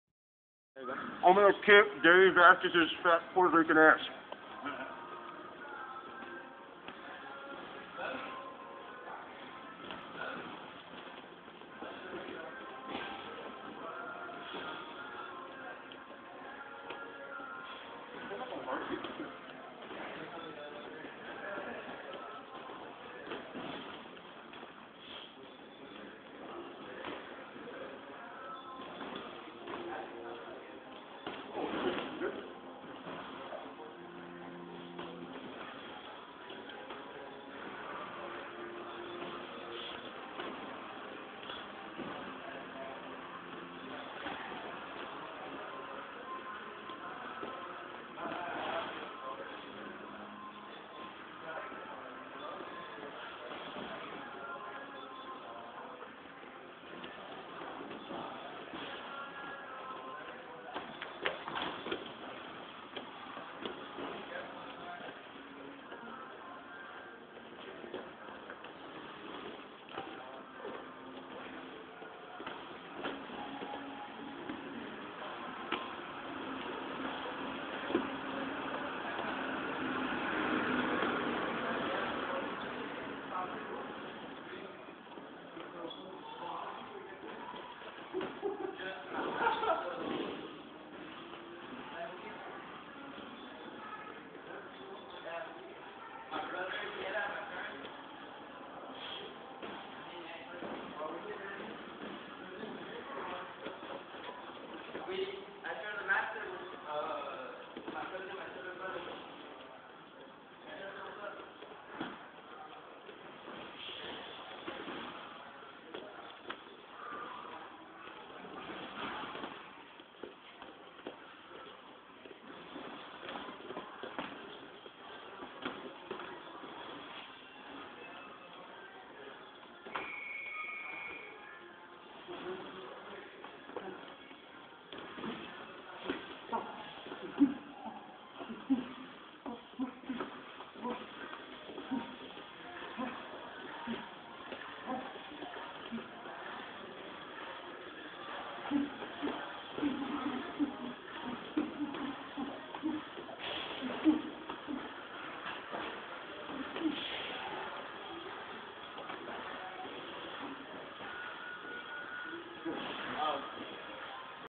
Btp battle of big belly crew Joey v dave
BTP Sparring exercise for the big boys